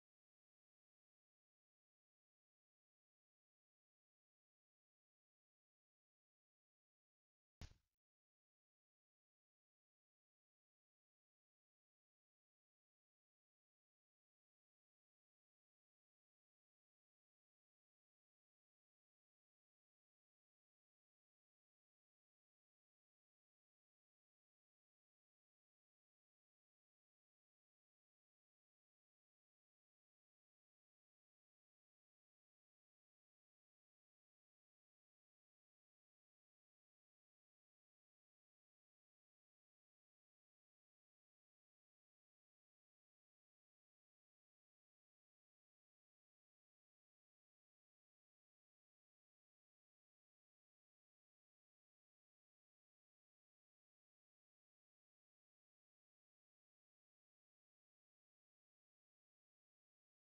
EFRS Male Marsh Harrier
Shown at half speed. Male Marsh Harrier hunts the reed beds